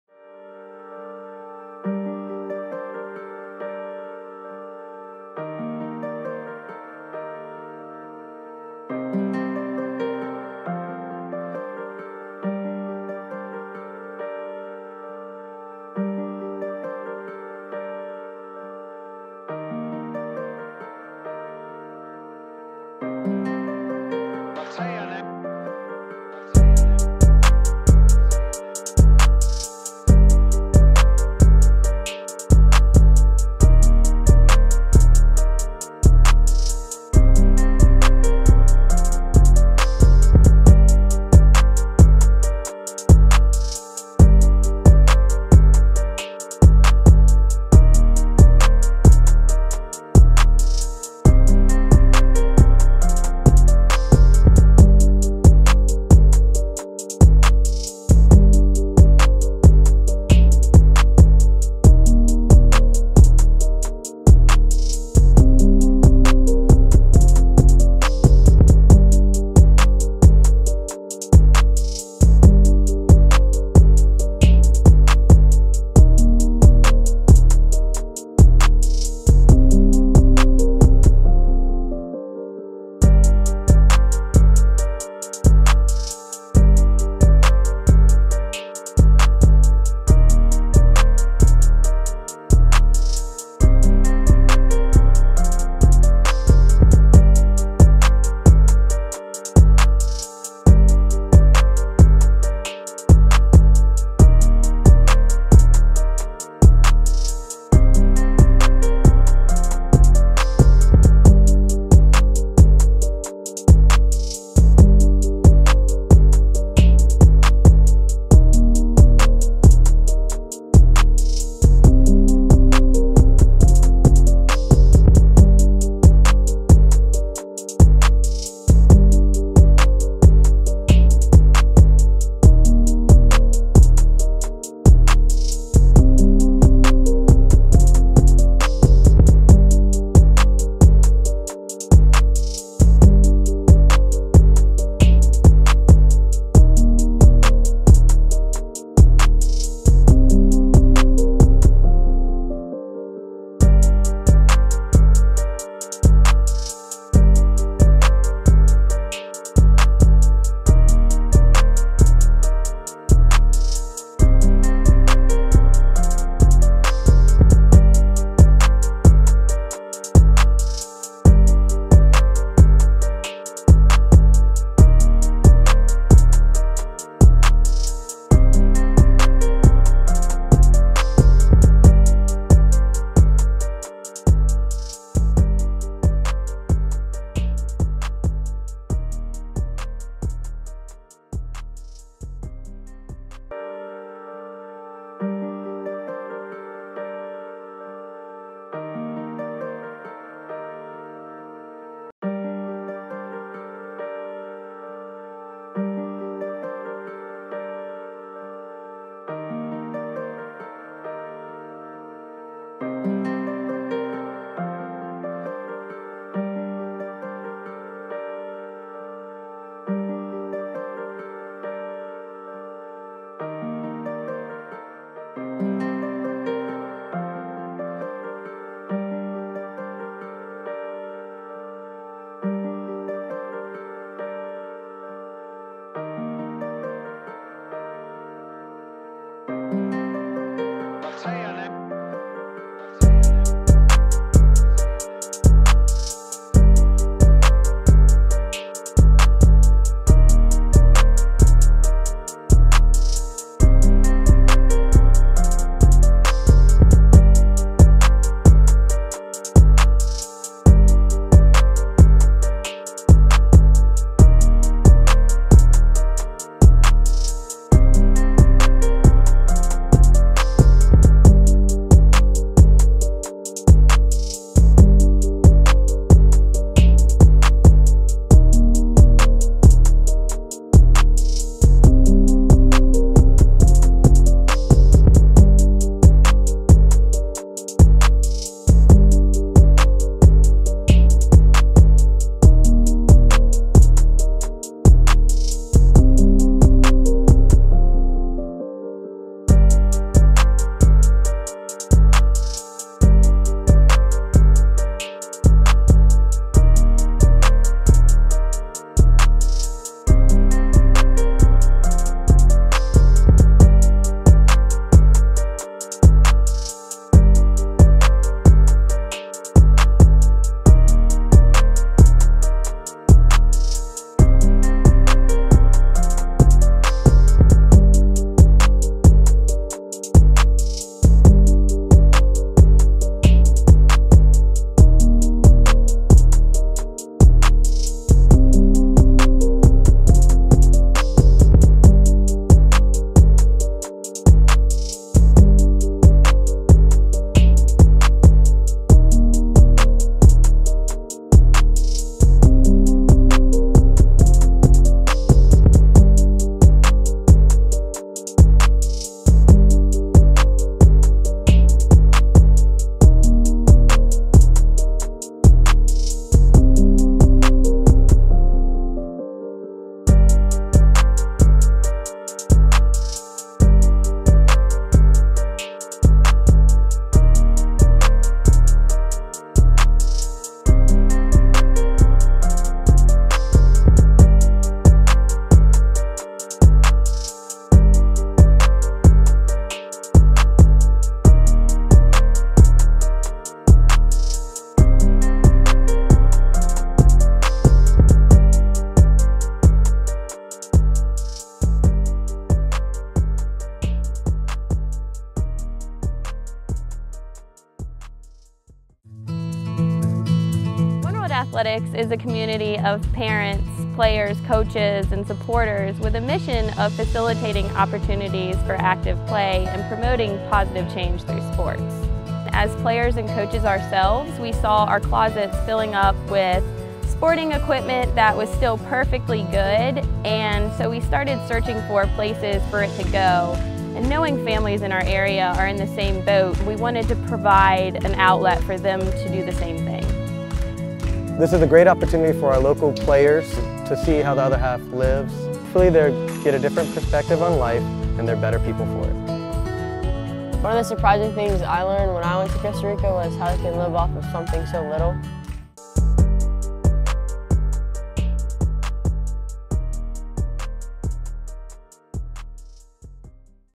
Joshua Glade Soccer Highlight Reel (Class of 2021)
This video captures various clips of Joshua Glade in both Club and High School soccer games.  Great for coaches looking to assess recruits at the college level.